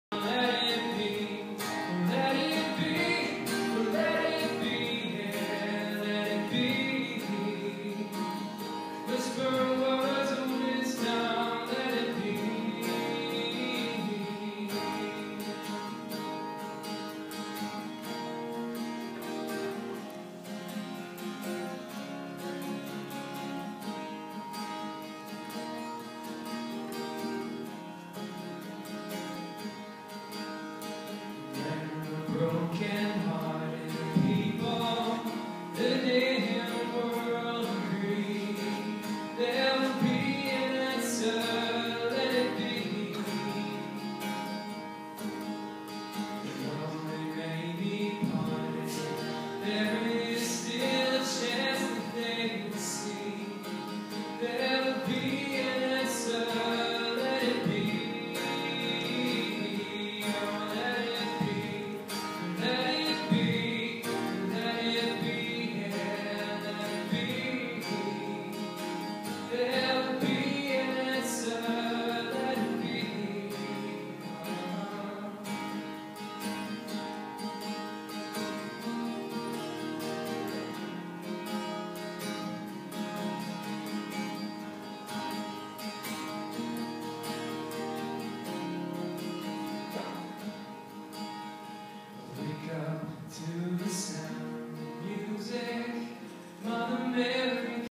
Greg Schroeder at Pacific's Got Talent 2013
Greg Schroeder playing and singing at Pacific's Got Talent